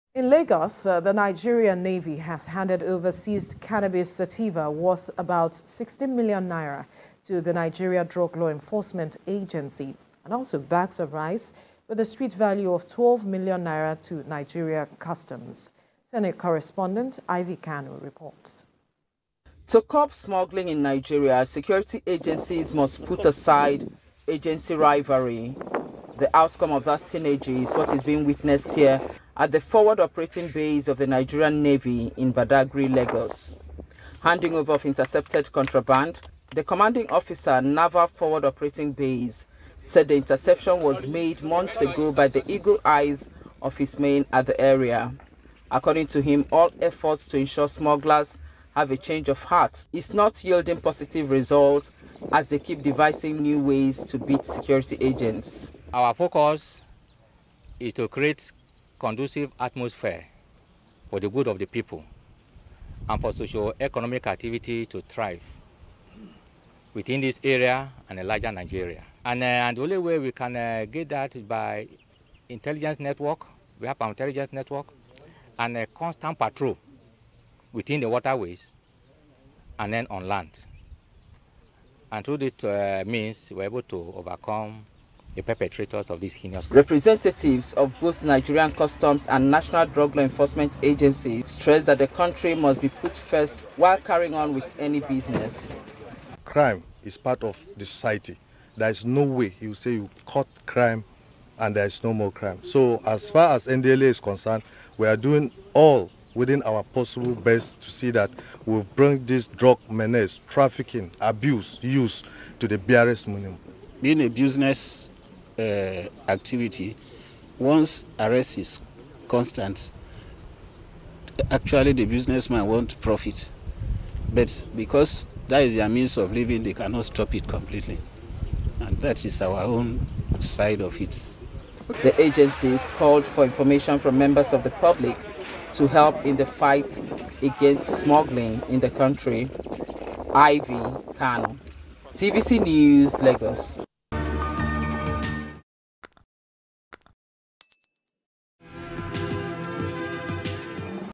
Synergy Is The Only Way To Beat Smugglers - Security Agencies
The Nigerian Navy has handed over seized Cannabis Sativa worth about 16 million naira to the Nigeria Drug Law Enforcement Agency and also bags of rice with the street value of 12 million naira to Nigerian customs.